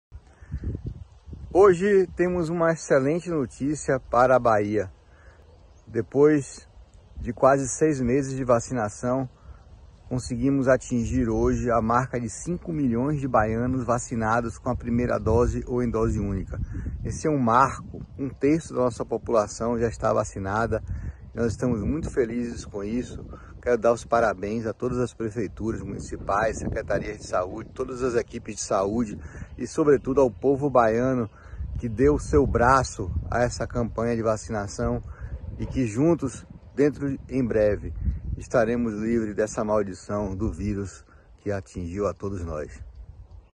Vídeo do secretário de Saúde da Bahia sobre a marca de 5 milhões de vacinados
Vídeo de Vitor Fernandes 🚨